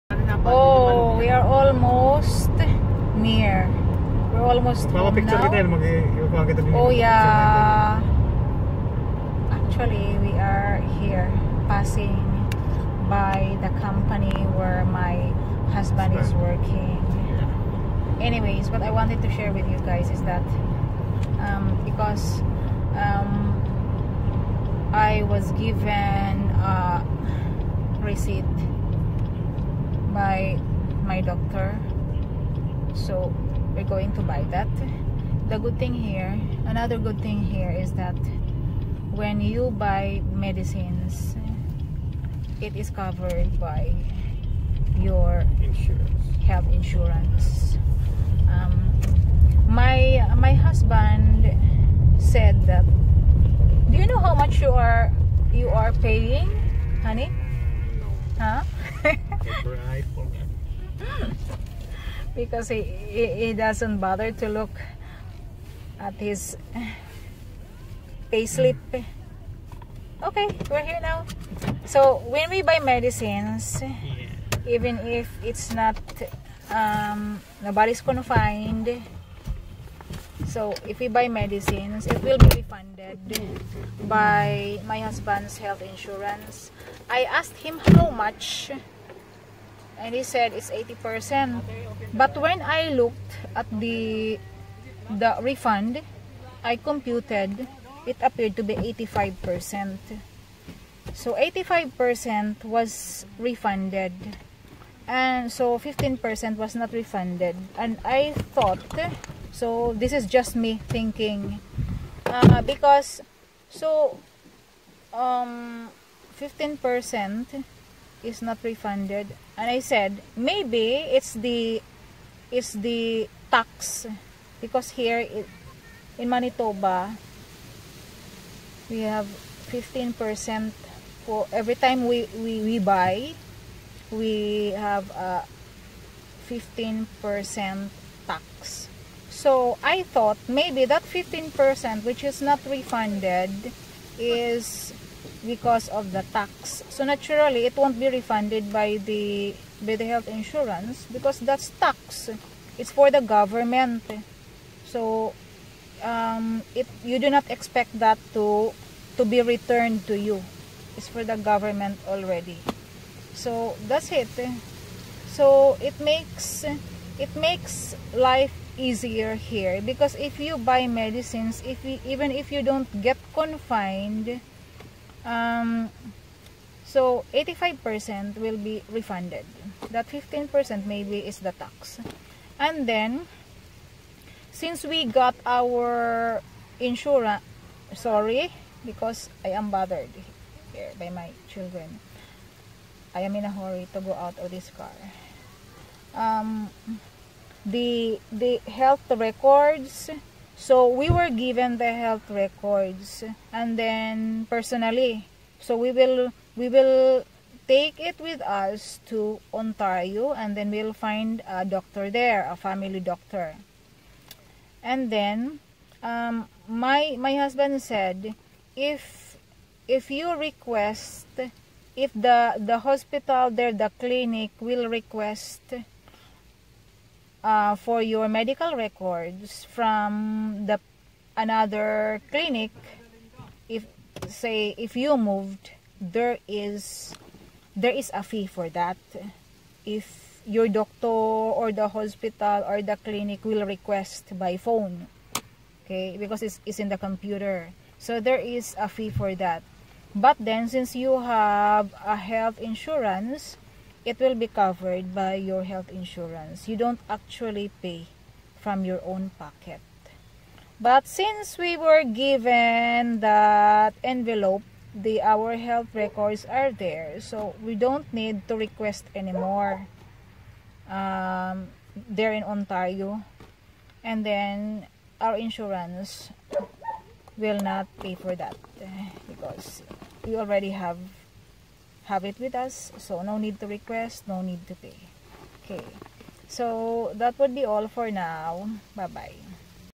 healthcarePinoy Family in Canada - We Get a 100% Refund of Our Prescribed Medicines as Out-Patients
It's a 100% refund. It appears to be 85% because there is a 15% tax. This time I read his health care policy and it said there's a 100% refund on most medicines, and my husband receives a 100% refund everytime he buys medicines.
To make my point clear, if you buy medicines worth 100 CAD + 15% tax, you'll be paying 115 CAD. You will get a refund worth 100 CAD because that's what the medicines are worth. It only says 100% refund on most medicines, it does not refund the sales tax, naturally, because it shouldn't be returned to you because it's for the government already, and even if it is returned to you, it is in the form of services by the government. It could  be even more than what you paid for.
Those medicines that are not refunded were not soecified by the health insurance.

#100%refundonmedicines